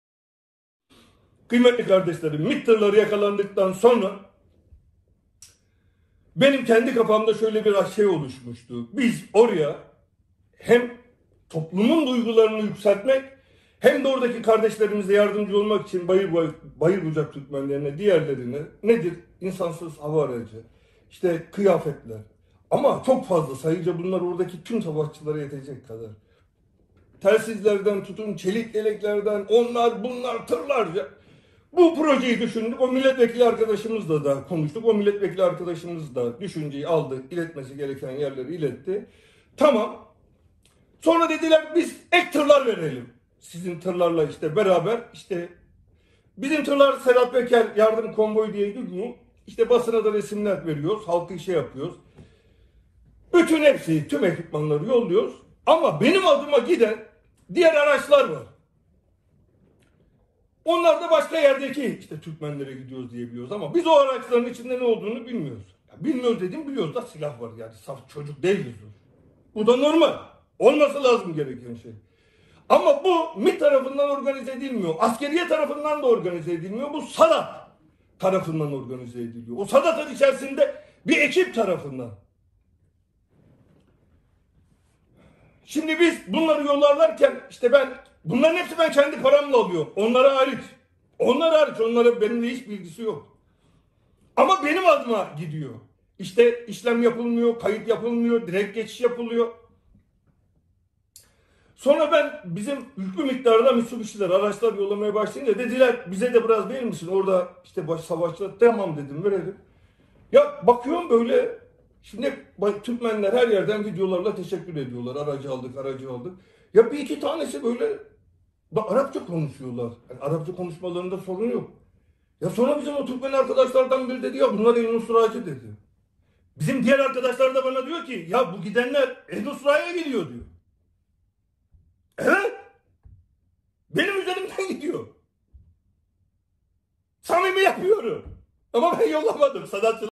Sedat Peker 8. bölümde MİT tırlarıyla ilgili müthiş iddia
Sedat Peker 8. bölümde MİT tırlarıyla ilgili müthiş iddia: "Benim adıma giden tırlarda SADAT tarafından El Nusra cihatçılarına tonlarca silahlar gönderildi."